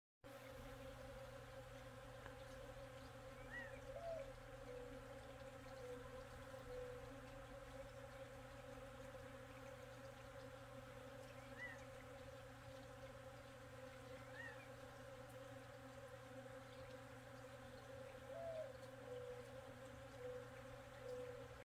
Bees 05 12 2023
Bees in blooming Mesquite tree, Redington Ranch, Tucson, Arizona